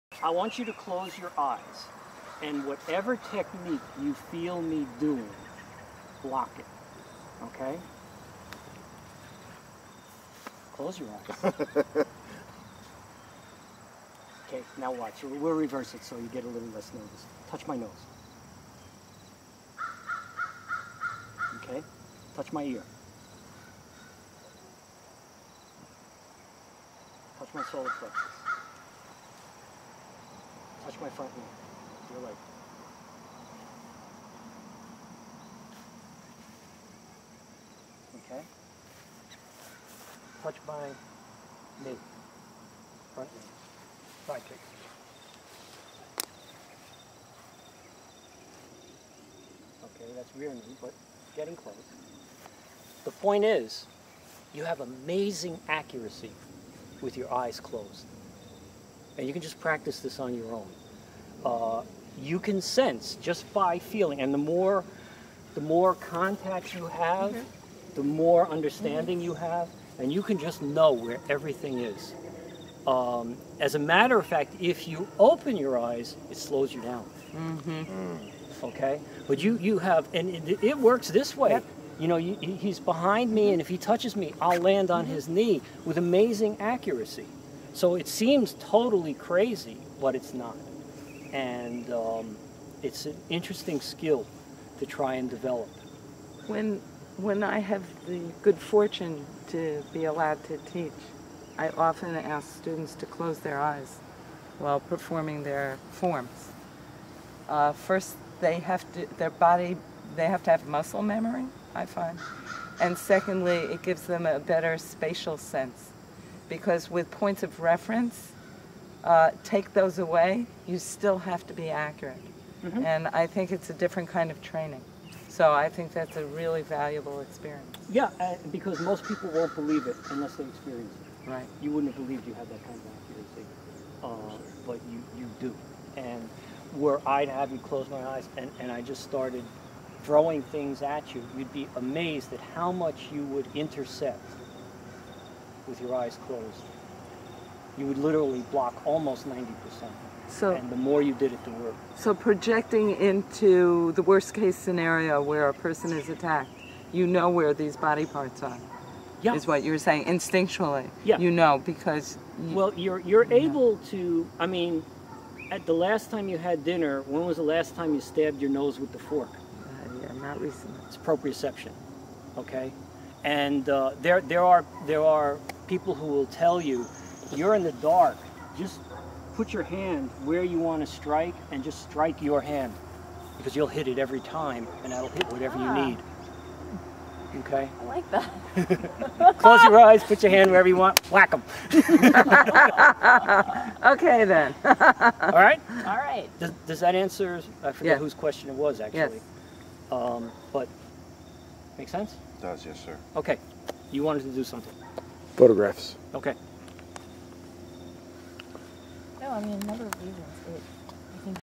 closed eye techniques
fighting by "feeling" (ie, proprioception) without the benefit of eyesight